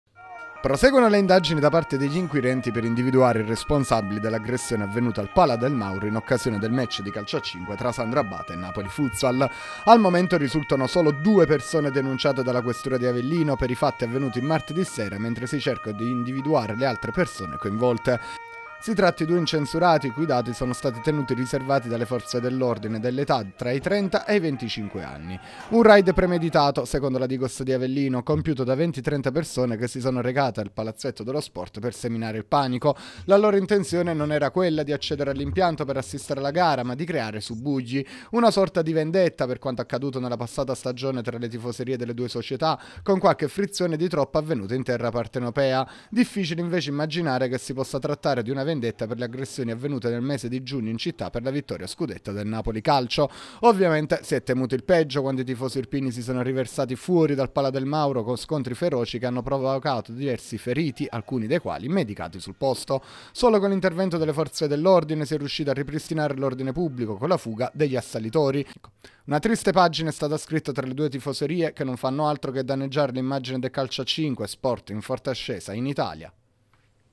Scontri al PalaDelMauro: due tifosi napoletani denunciati
Due persone denunciate per gli scontri avvenuti, al Paladelmauro, tra i tifosi del Napoli Futsal e quelli della Sandro Abate.